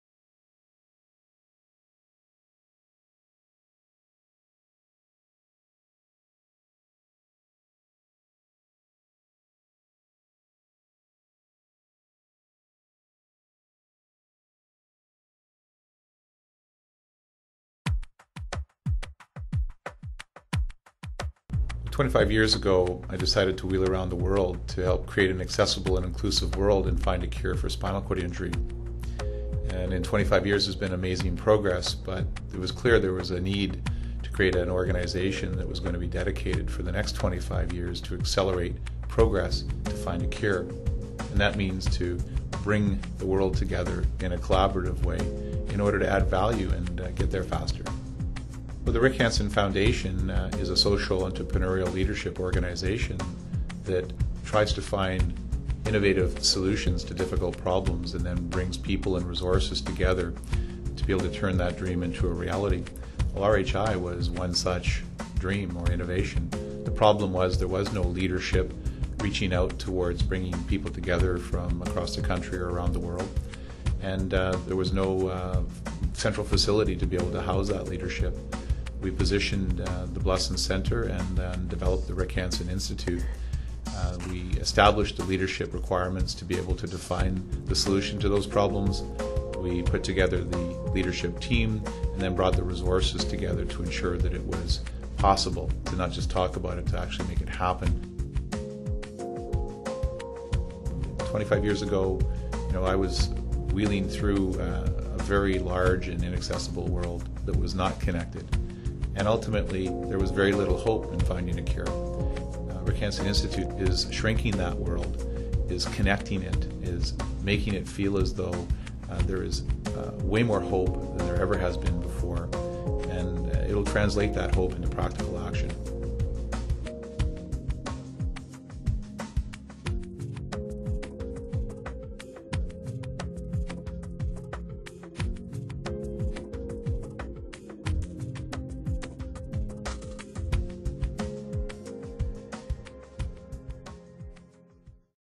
The Story Behind the Rick Hansen Institute
The Rick Hansen Institute is now Praxis Spinal Cord Institute. Praxis exists to lead global collaboration in spinal cord injury (SCI) research, innovation, and care. By using SCI knowledge translation to bridge health evidence with real world delivery, we work to make exceptional improvements in the health of people living with SCI.

We bring together researchers, health care professionals, service providers and people with spinal cord injury (SCI), all committed to improving outcomes for people with SCI -- through research, infrastructure, knowledge exchange and the identification and implementation of best practices.

The Institute is designed to influence all elements of the innovation system for SCI treatment, care, and other services, including developing new therapies and decreasing the time for research to be translated into real-life benefits. RHI does this by leading collaboration between researchers, clinicians, funding agencies, government and non-governmental agencies nationally, and, increasingly, internationally.